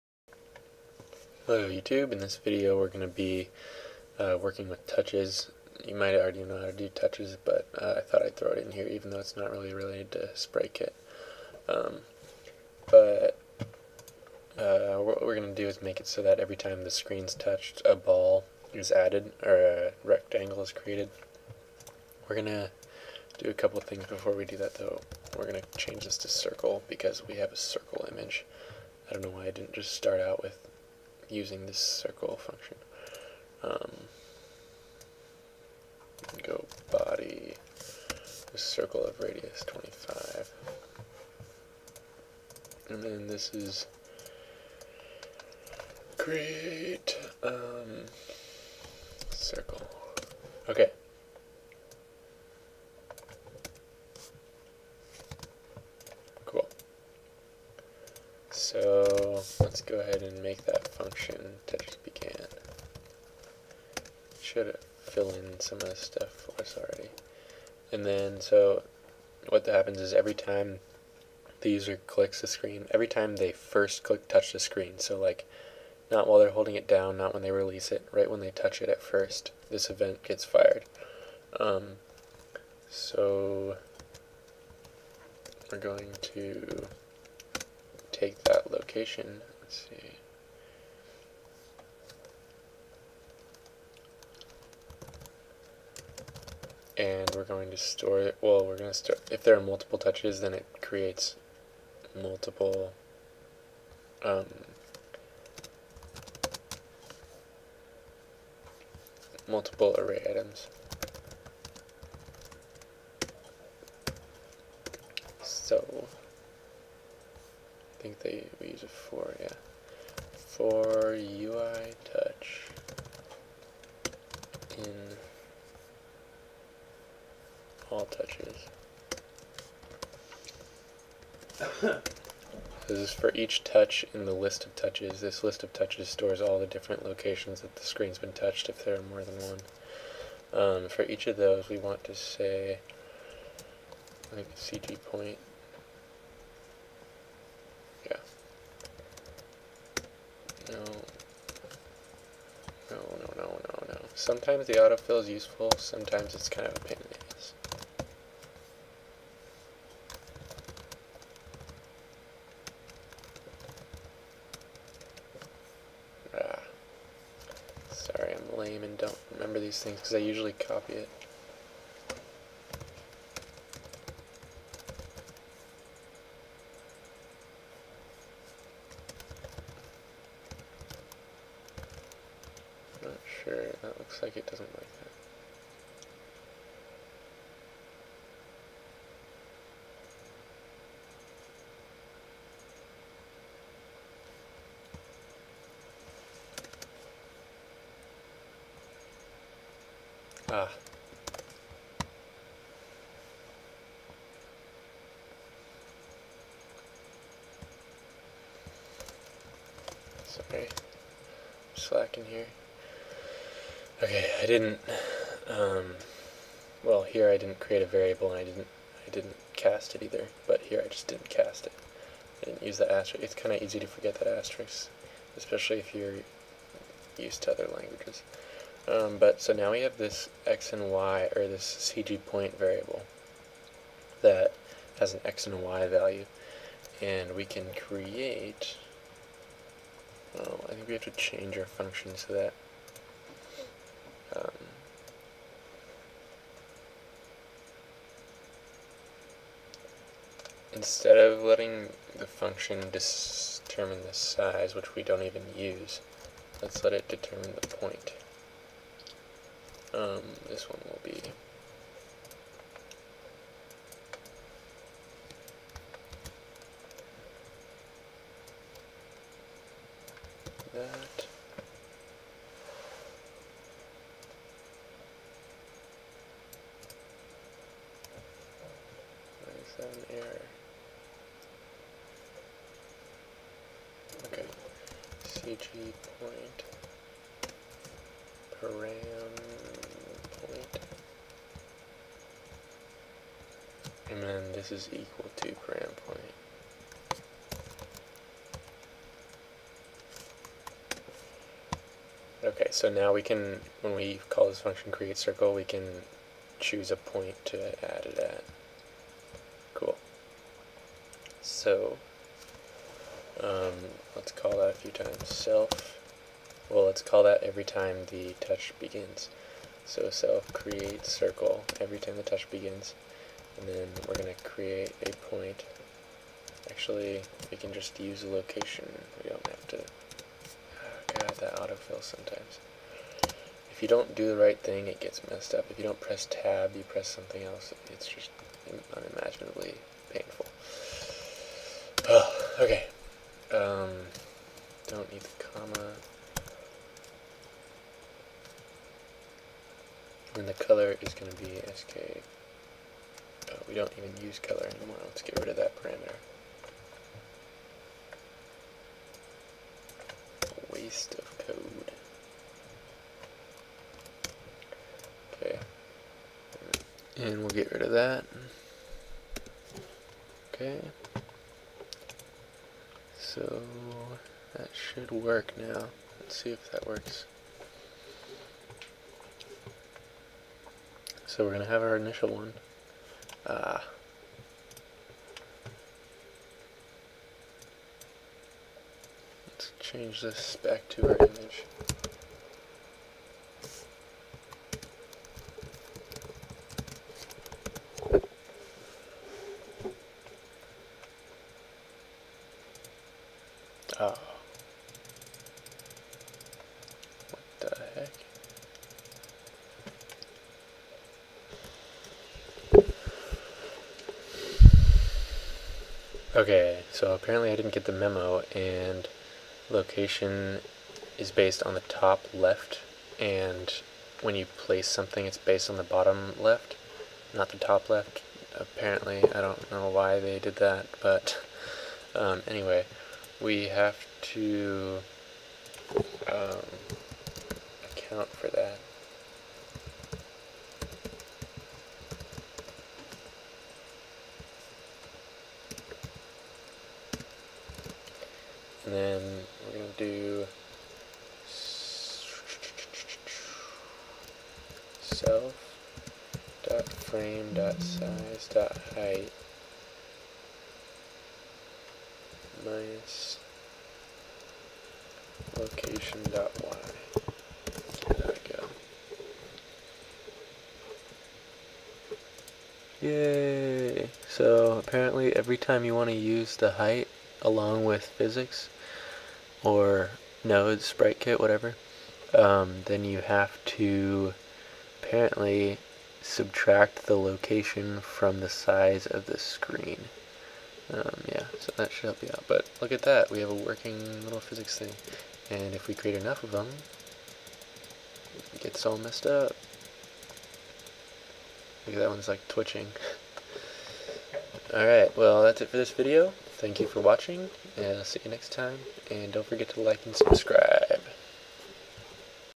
touchBegan Event - Xcode SpriteKit Tutorial 6
The 6th video in our Xcode SpriteKit Tutorial series.

In this video we use touchesBegan to create circles at the touches location.